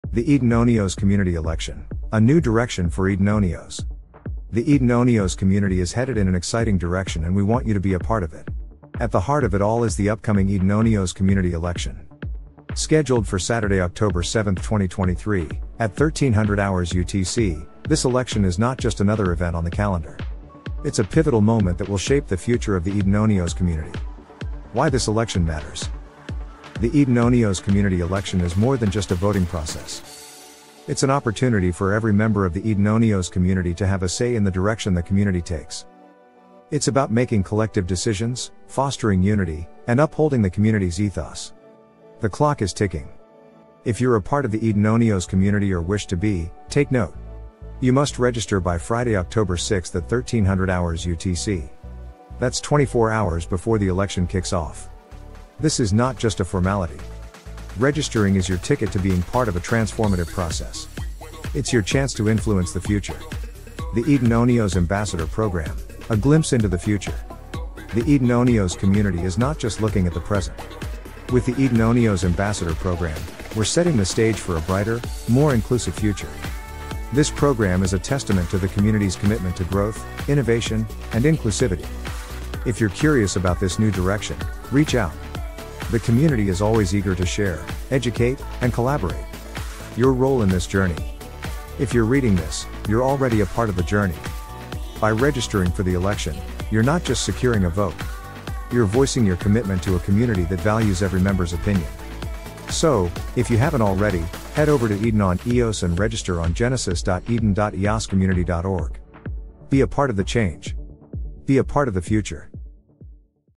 Ad. Eden On EOS Election Term VIII
Help grow the ecoystem in a meaningful way
by taking part in the Eden on EOS elections Term VIII

Sign up is now open until October 6 at 13:00 UTC

The EdenOnEOS Community Election: A New Direction for EdenOnEOS

The EdenOnEOS community is headed in an exciting direction and we want YOU to be a part of it! At the heart of it all is the upcoming EdenOnEOS Community Election.

Scheduled for Saturday, October 7th, 2023, at 13:00 UTC, this election is not just another event on the calendar.

It's a pivotal moment that will shape the future of the EdenOnEOS community.

Why This Election Matters

The EdenOnEOS Community Election is more than just a voting process.

It's an opportunity for every member of the EdenOnEOS community to have a say in the direction the community takes.

It's about making collective decisions, fostering unity, and upholding the community's ethos.

If you're a part of the EdenOnEOS community or wish to be, take note:

*You must register by Friday, October 6th at 13:00 UTC*

That's 24 hours before the election kicks off. This is not just a formality.

Registering is your ticket to being part of a transformative process. It's your chance to influence the future.

The EdenOnEOS Ambassador Program: A Glimpse into the Future

The EdenOnEOS community is not just looking at the present. With the EdenOnEOS Ambassador program, we're setting the stage for a brighter, more inclusive future.

This program is a testament to the community's commitment to growth, innovation, and inclusivity.

If you're curious about this new direction, reach out. The community is always eager to share, educate, and collaborate.

Your Role in This Journey

If you're reading this, you're already a part of the journey.

By registering for the election, you're not just securing a vote. You're voicing your commitment to a community that values every member's opinion.

So, if you haven't already, head over to Eden on EOS and register on genesis.eden.eoscommunity.org. Be a part of the change. Be a part of the future.